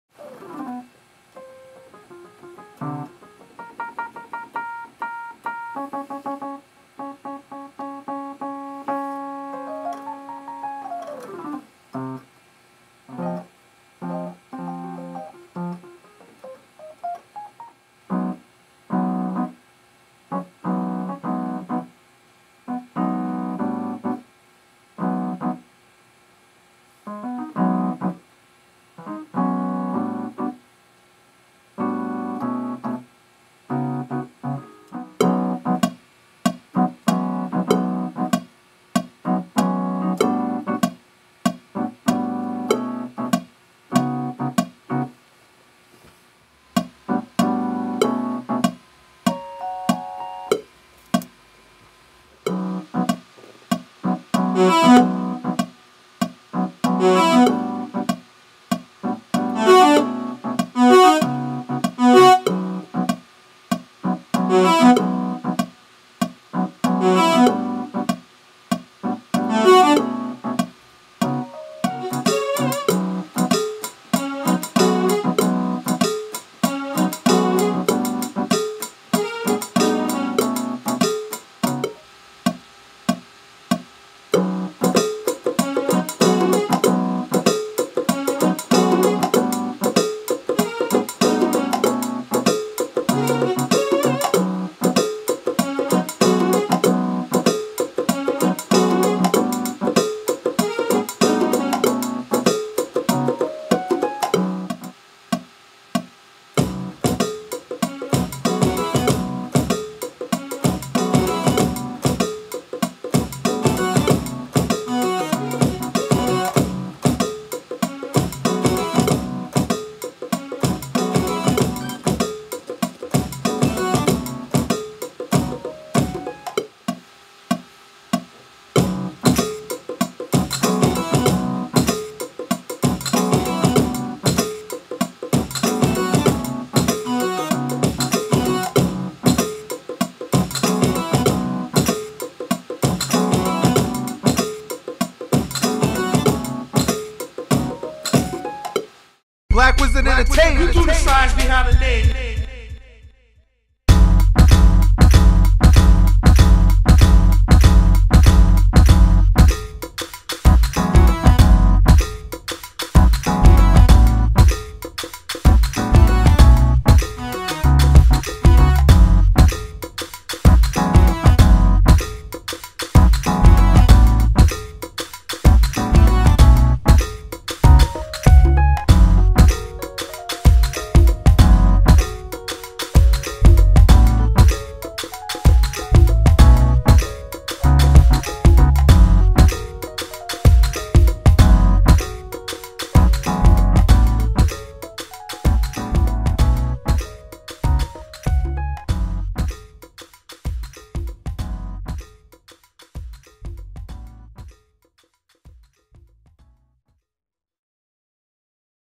Pa-Zaz Makes a Beat, Vol.2
Just got home for winter break and what better way to kick things off than to make a new beat!